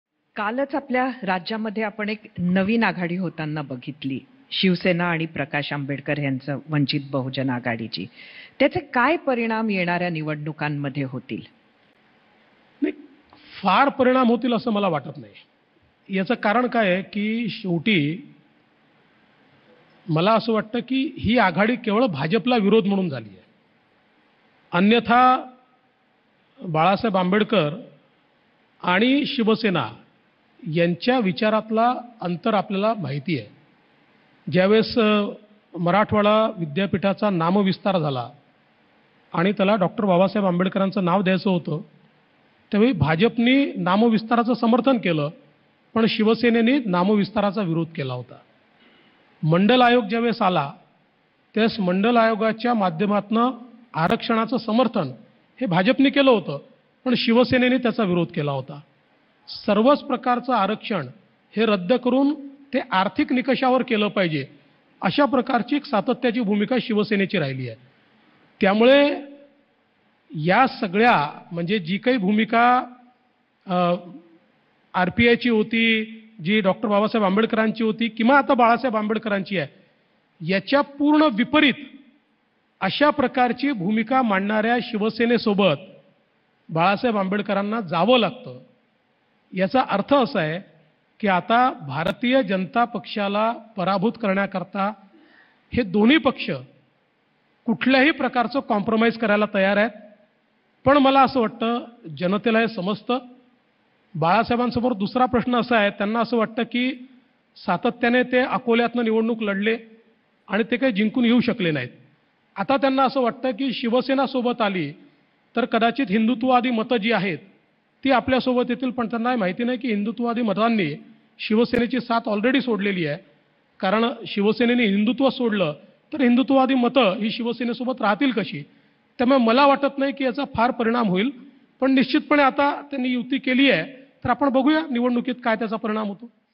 उद्धवजी ठाकरे आणि वंचितची आघाडी केवळ भाजपला विरोध करण्याकरीता! | मुंबई | DCM Devendra Fadnavis
MajhaMaharashtraMajhaVision | 24/01/23